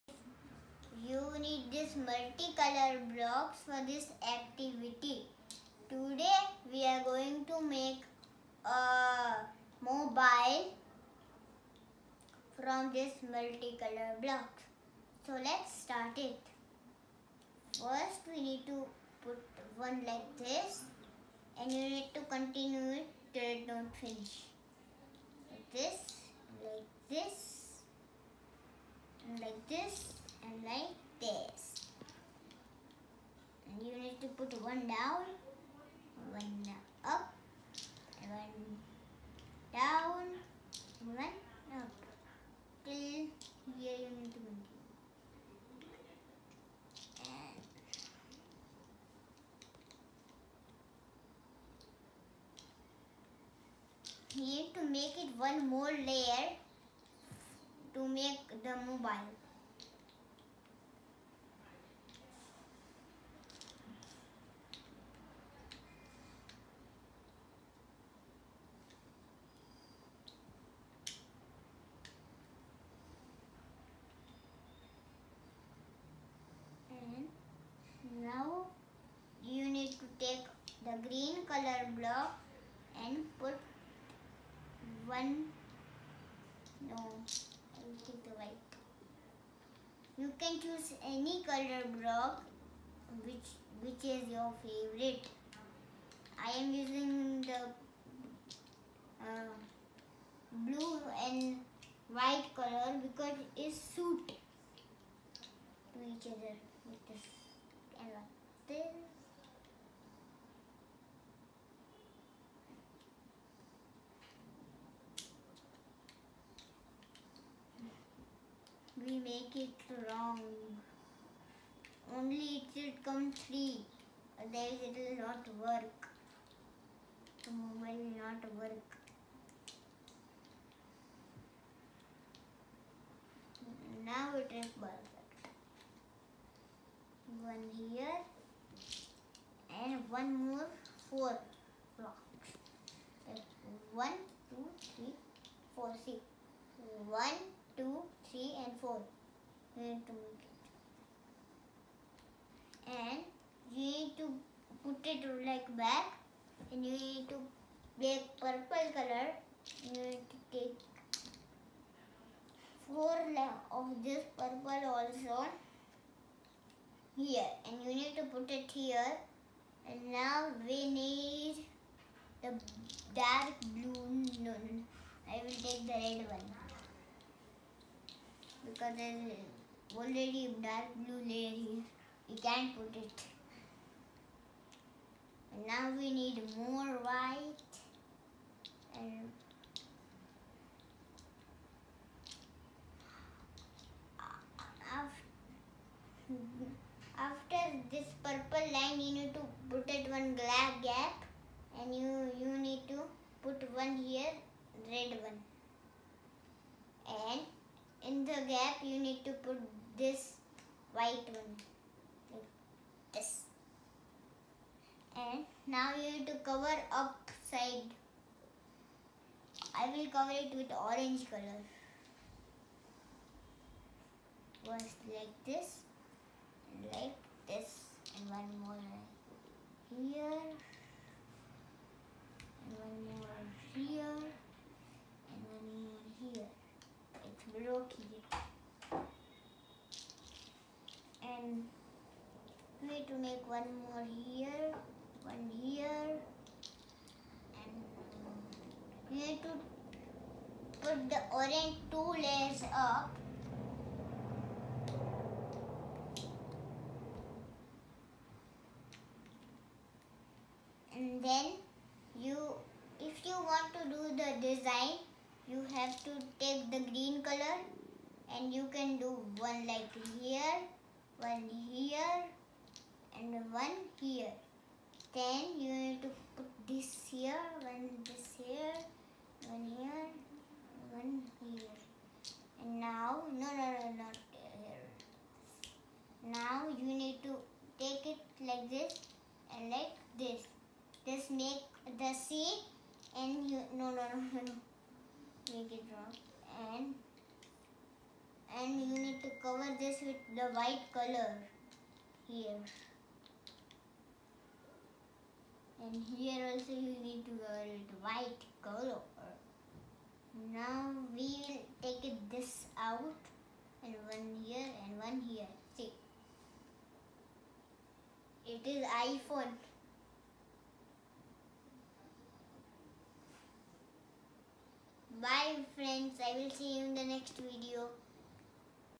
Mobile Creation using multi-coloured Blocks
Mobile has become so essential in our routine life that even a child understands its importance.  Here we show how to create an eco-friendly mobile using the multi-coloured blocks that does not require electricity or chargers, but will provide you all the entertainment.

Mobile | Eco-friendly | Multi-coloured blocks | life | Essential